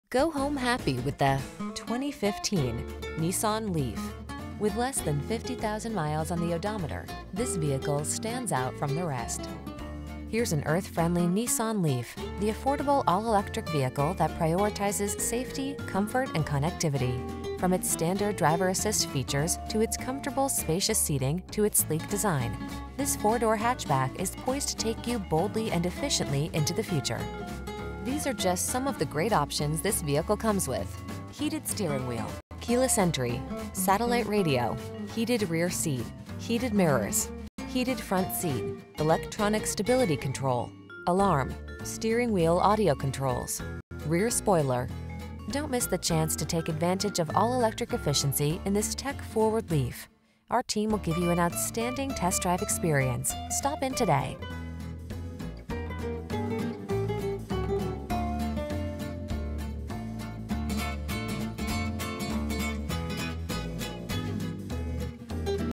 2015 Nissan Leaf Kennesaw, Marietta, Smyrna, Woodstock, Atlanta, GA P7758
Pearl White Used 2015 Nissan Leaf S available in Kennesaw, Georgia at Town Center Nissan. Servicing the Marietta, Smyrna, Woodstock, Atlanta, GA area.

2310 Barrett Lakes Blvd NW